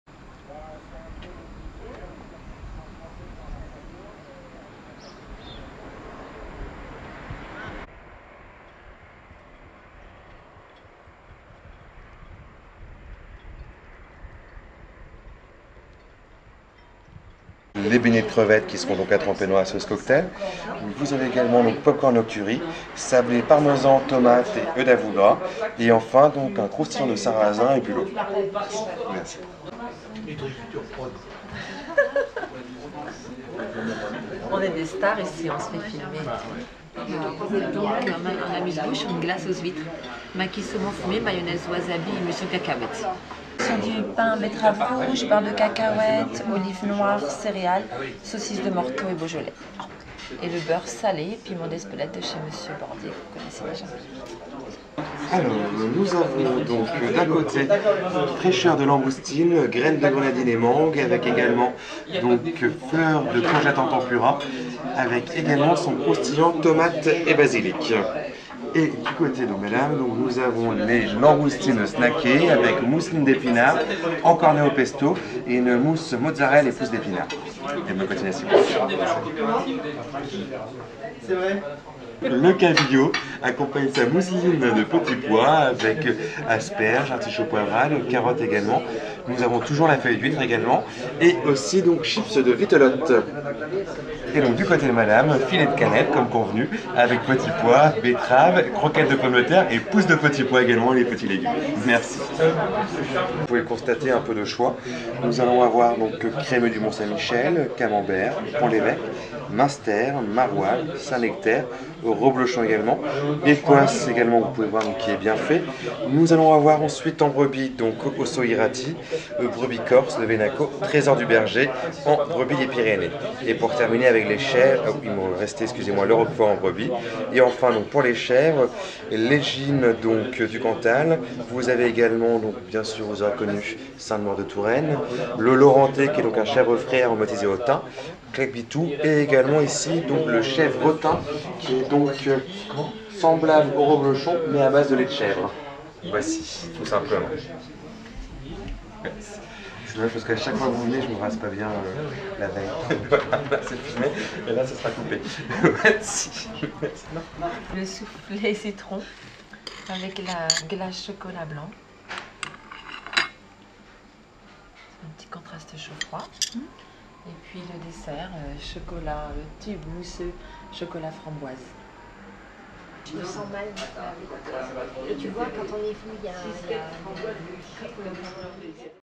Déjeuner à la Vieille Tour * - 27 juin 2014
C'est toujours avec un grand plaisir que je fais escale dans cette Vieille Tour. Que ce soit la cuisine ou l'accueil, tout ici n'inspire que confiance et satisfaction. Cerise sur le gâteau, au niveau tarifaire, ce restaurant est certainement le meilleur rapport qualité/prix breton dans le monde des étoilés.